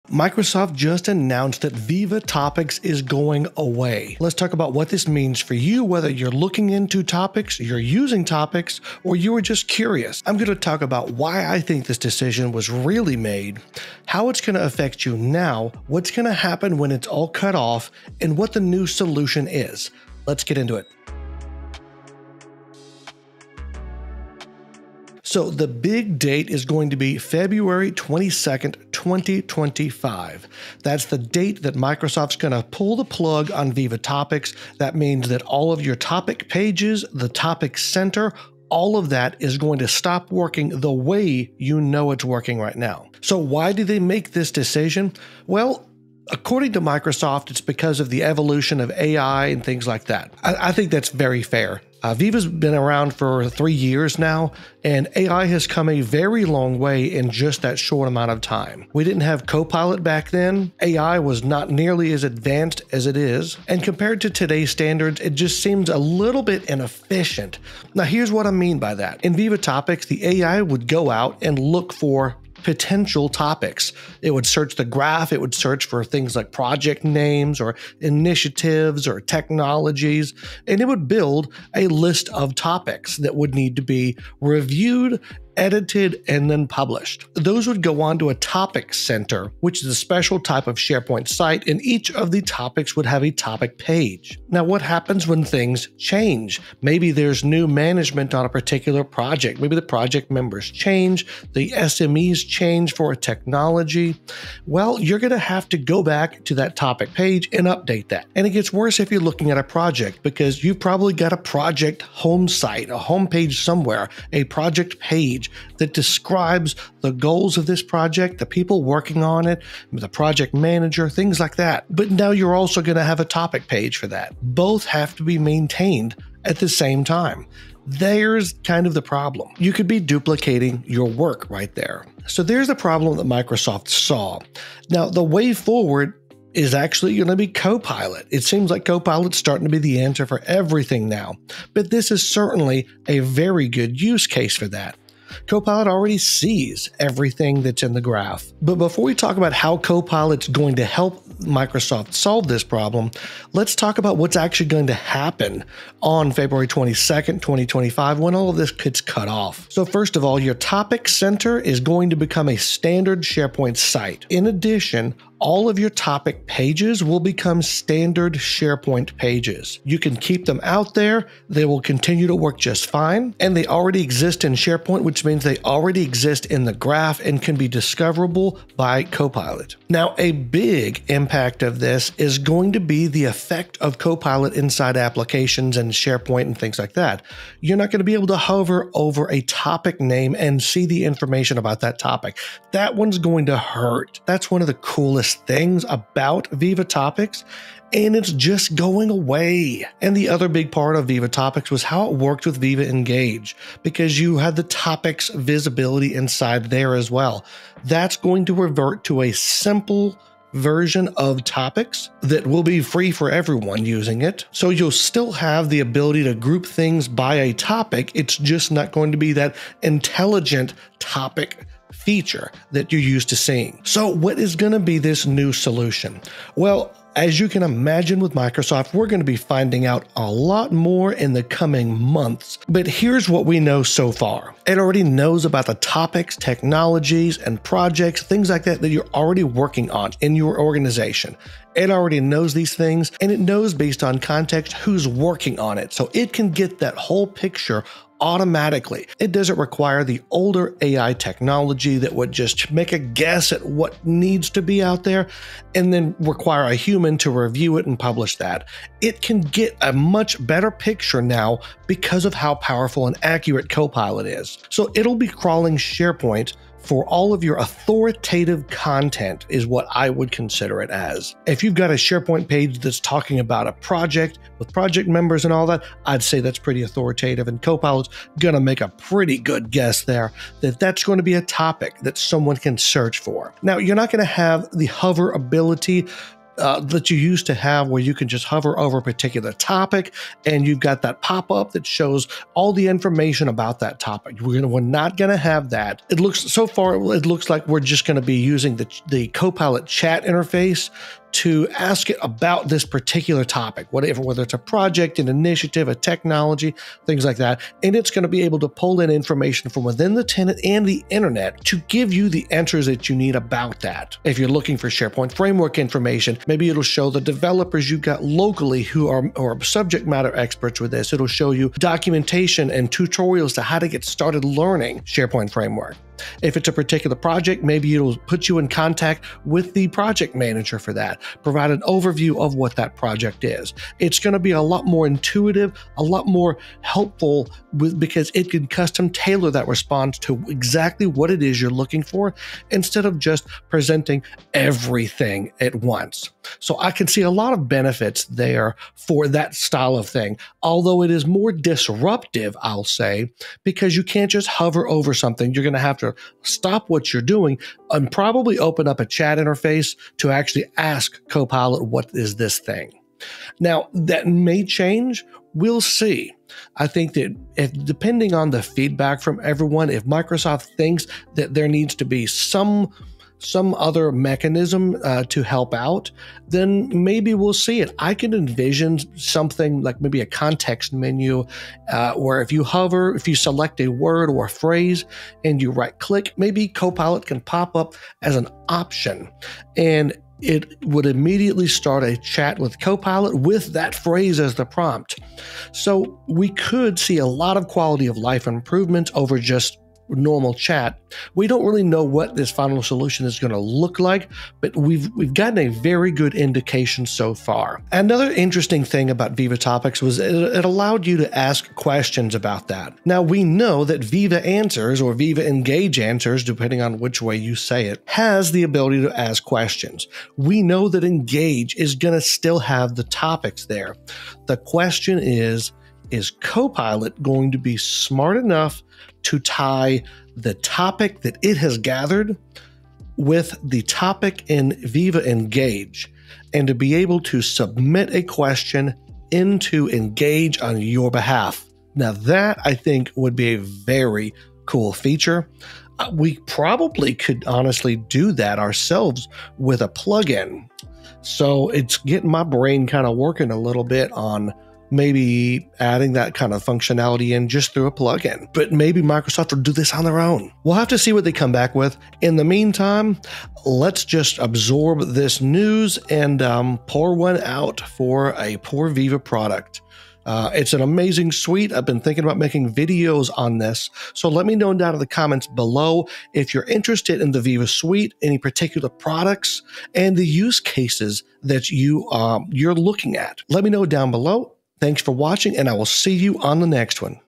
Retirement of Microsoft Viva Topics! We'll Miss You
Microsoft just announced that a major part of the Viva Suite is being retired. Let's talk about what this means for Viva Topics, what will happen, how it will affect you, and what the new solution is.

🆓 My guides, cheat sheets, and freebies

🔥 Get My Newsletter - SharePoint Insights🔥

📘 Looking for a great book on SharePoint Online?
I was a technical reviewer for this and HIGHLY recommend it!

⌛ Timestamps:
00:00 - Intro
00:25 - When is this happening?
00:43 - Why retirement?
02:12 - The way forward
02:33 - Will everything break?
03:43 - The new solution
05:46 - One issue I see
06:47 - The feature I want to see